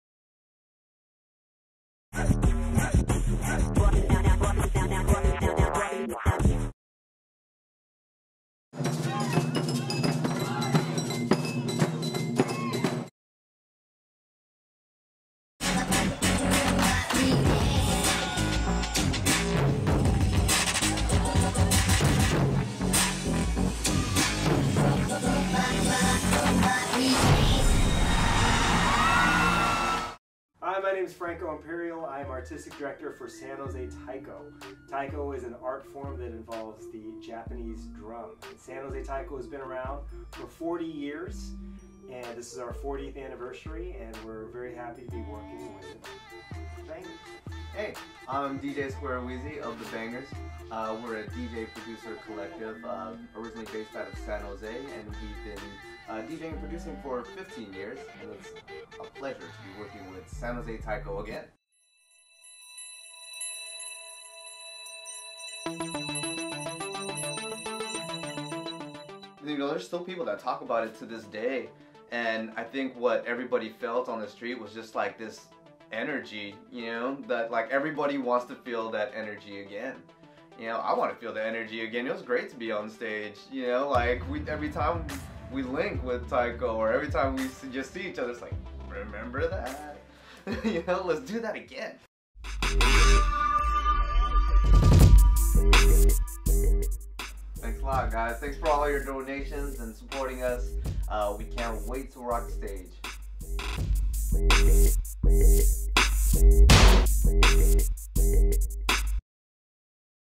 San Jose Taiko x The Bangerz in Concert
San Jose Taiko and DJ crew The Bangerz collaborate again since their first performance together the 2010 SubZero Street Festival.  The two groups have developed a concert program to be presented at the Montgomery Theater in downtown San Jose on June 15, 2013.